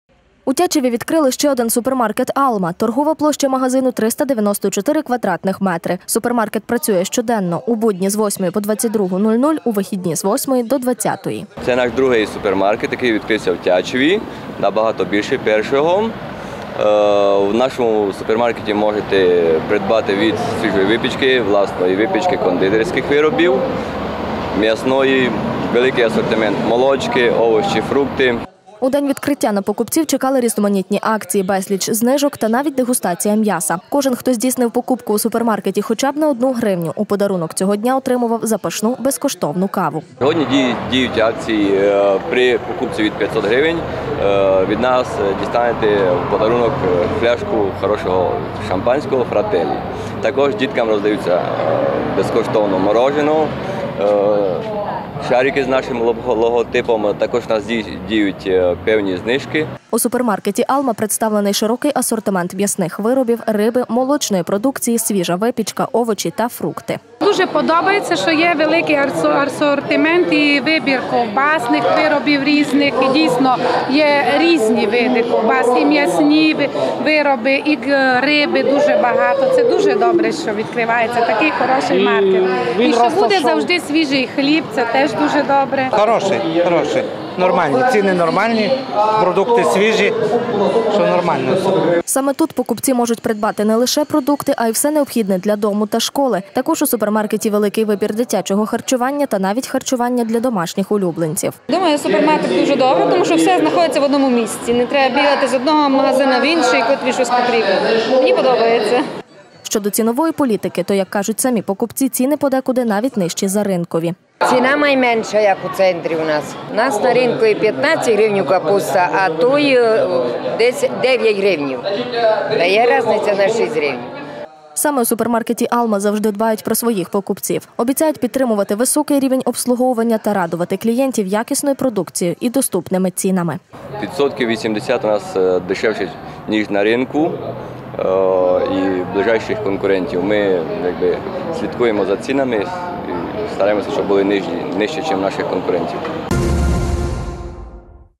Відкриття супермаркету
У місті Тячів днями відкрили ще один супермаркет «ALMA». Широкий асортимент продукції, помірні ціни, високий рівень обслуговування та постійні знижки та акції вже чекають на покупців.